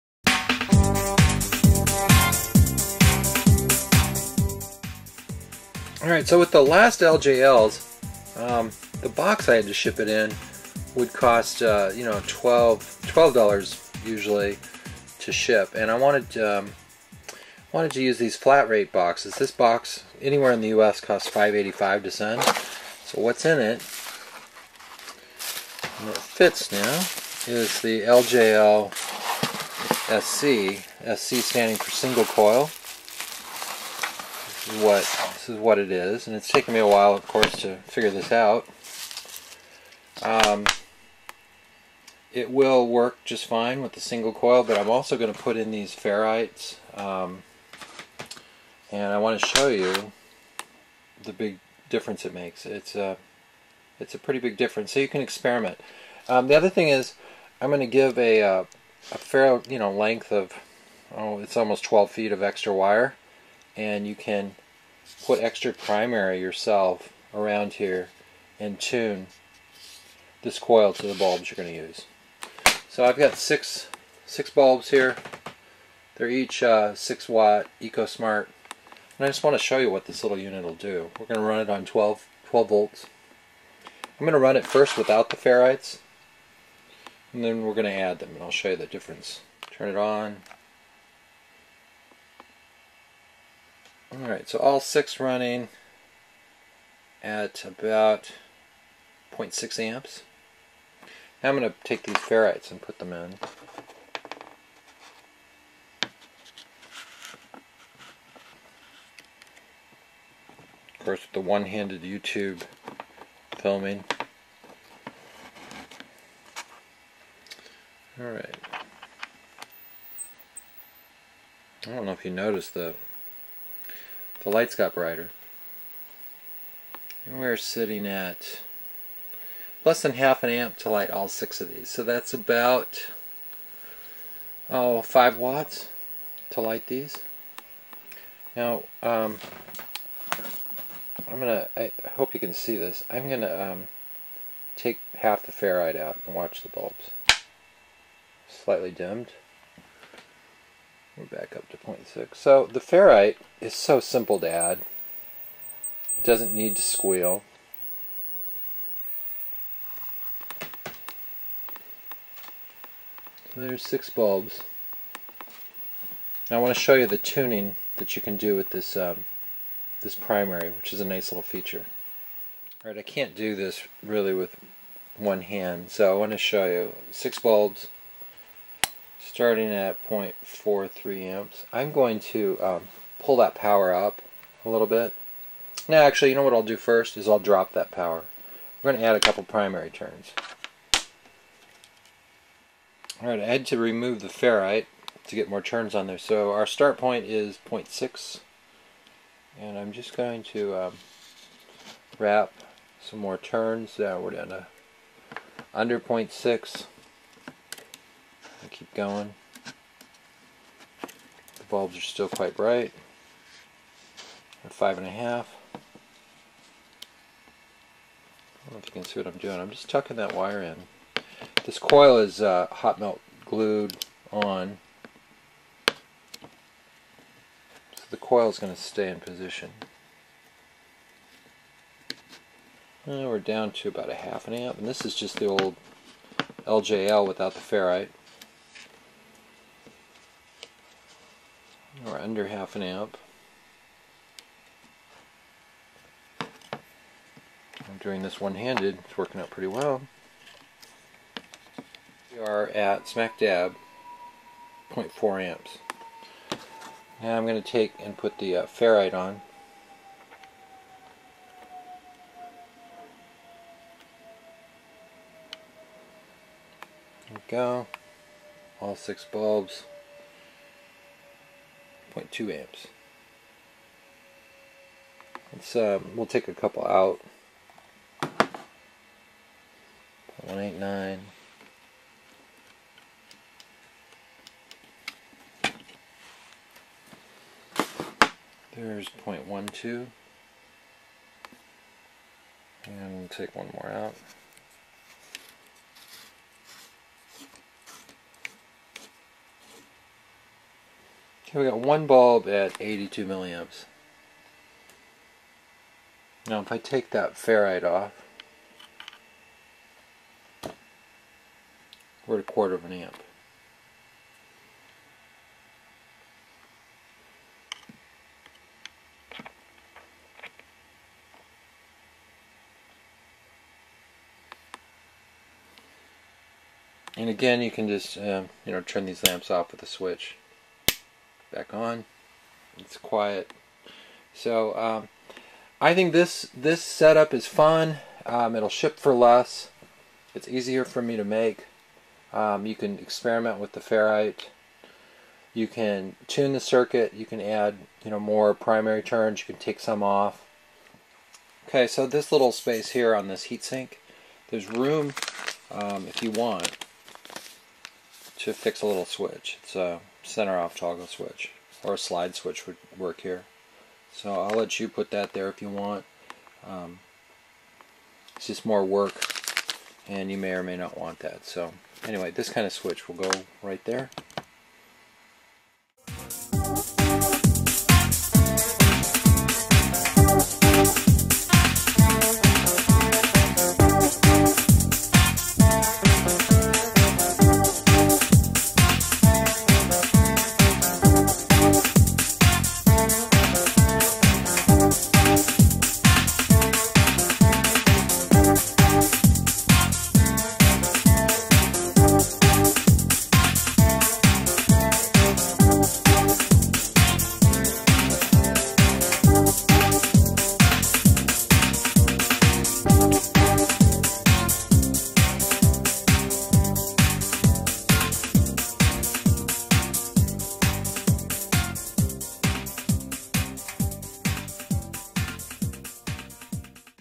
High performance LJL -SC
This is the result of months of testing and thinking of ways to make this simple circuit more robust, higher performance, smaller, cheaper to make and ship, and fun to tinker with.  It will light twelve or more 6 watt (40 watt equiv) LED bulbs.  Great for when the lights go out, camping, or as a add on to a solar panel.  I hope you like it.